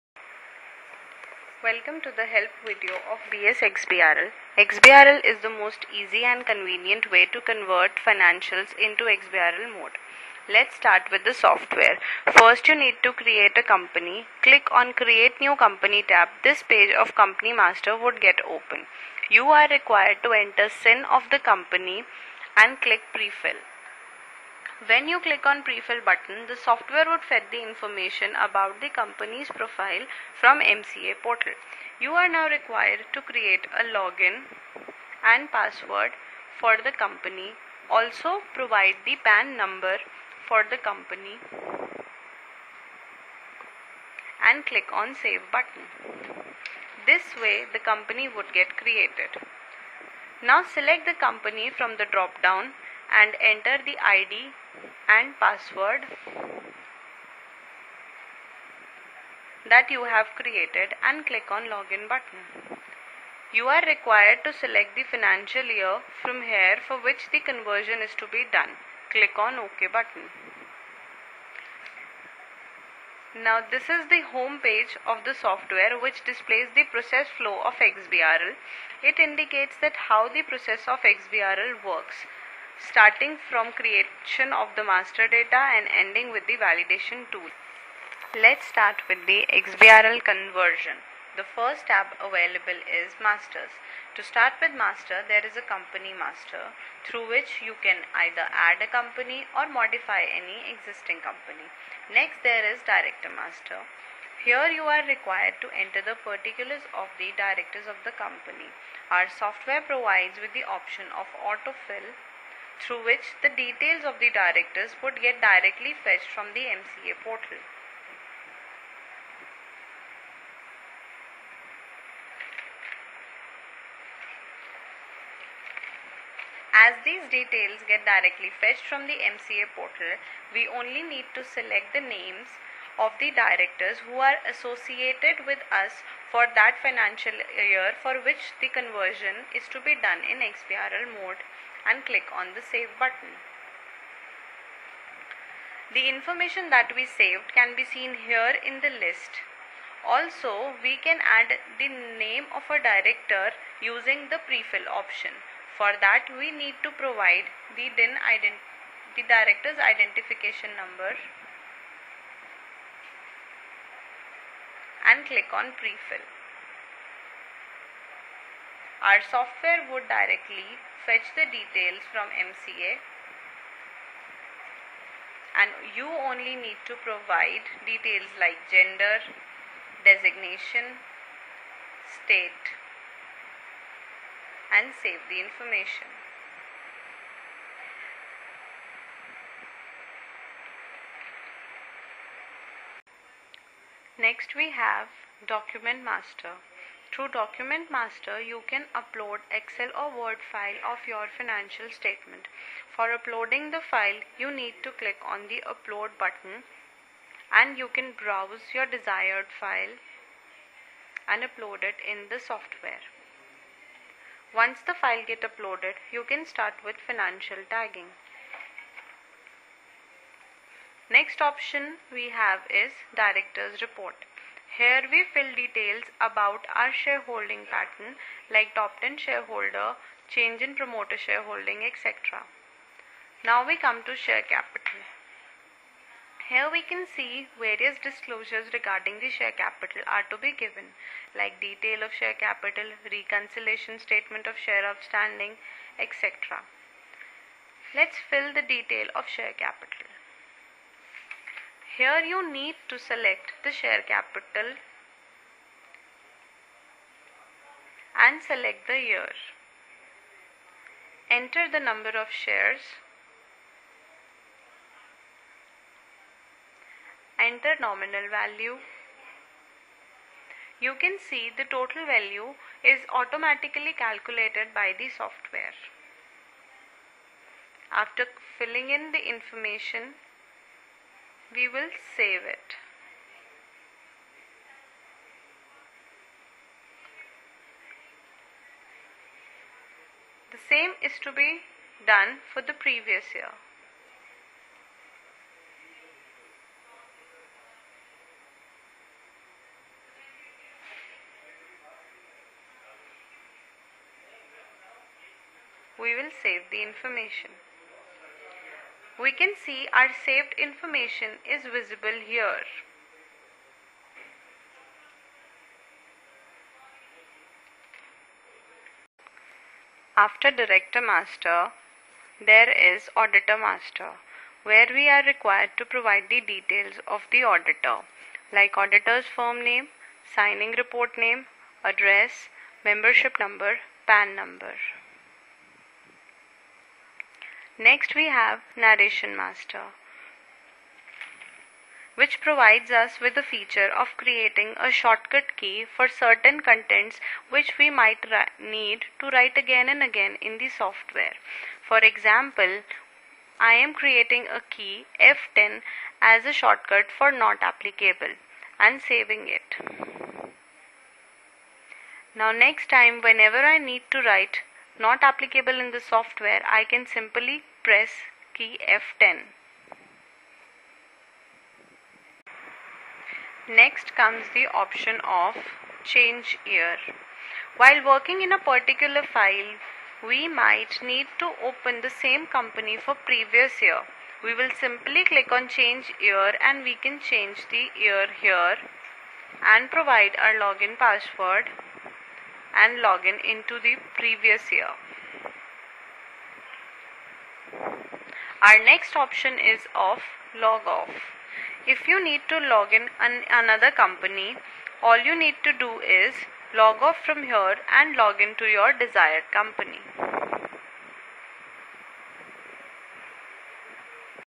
WEBTEL Help Video on XBRL Conversion - Creating Master
Webtel provides the help video of the Web XBRL software for smooth conversion of financials into XBRL mode. This video hereby is based upon the first step of working in WEB XBRL software i.e; creation of Master Data and other related functions.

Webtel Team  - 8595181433
Mr. Praveen Sharma (Delhi/NCR) - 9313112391
Mr. Maneesh Aggarwal (Rest of India) – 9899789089